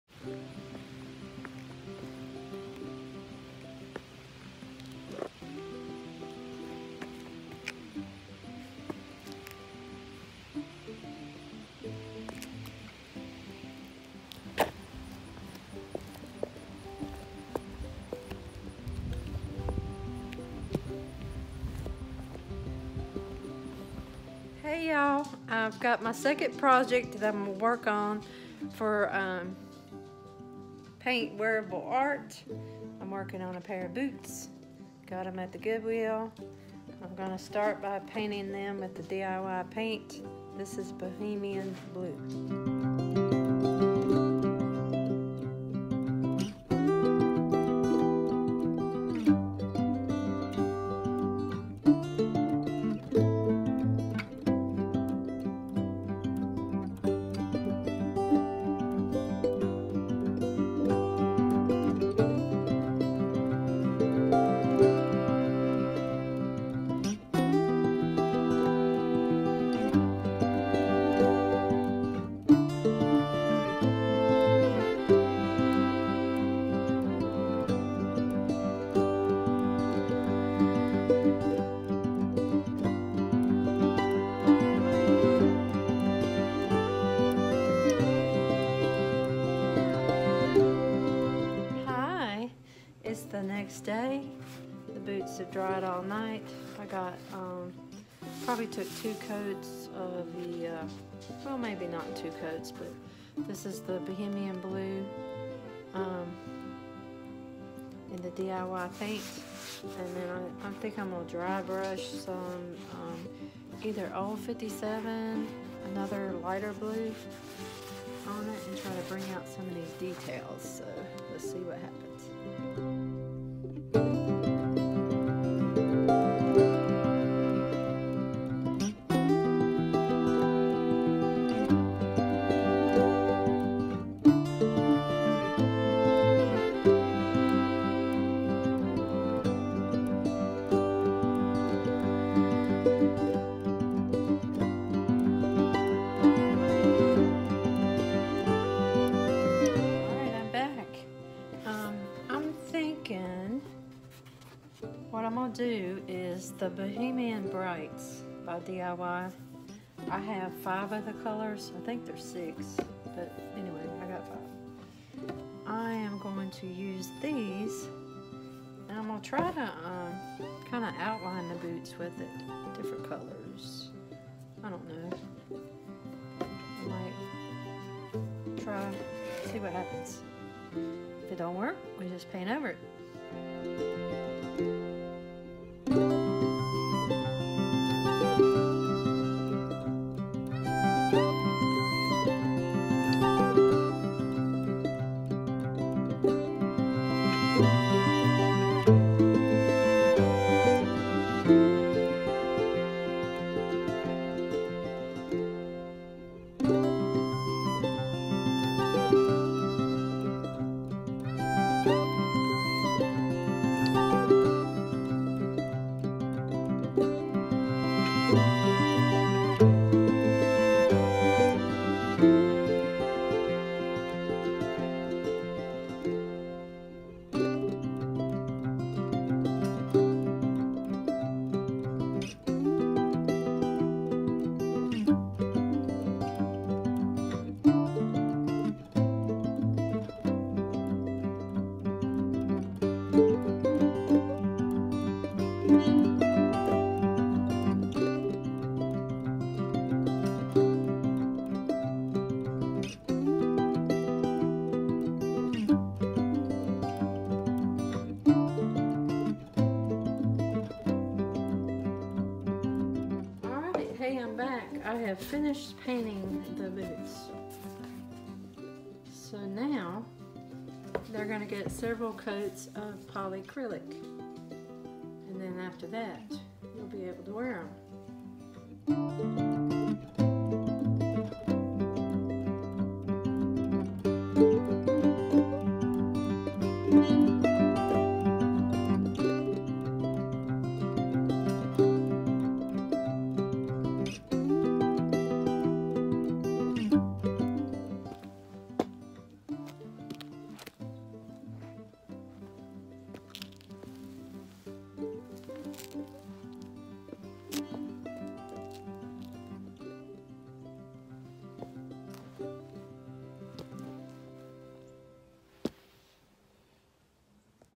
Paint a pair of boots with DIY paint
I painted a pair of boots with DIY paint for the DIY wearable art challenge put on by @TheTurquoiseIris